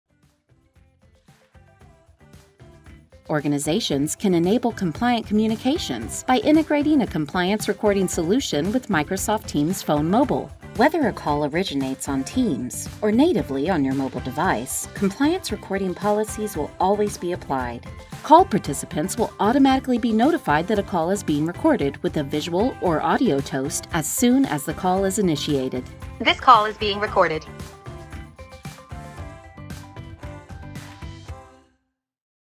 Integrate your compliance recording solution with Teams Phone Mobile
Organizations can enable compliant communications by integrating a compliance recording solution with Microsoft Teams Phone Mobile.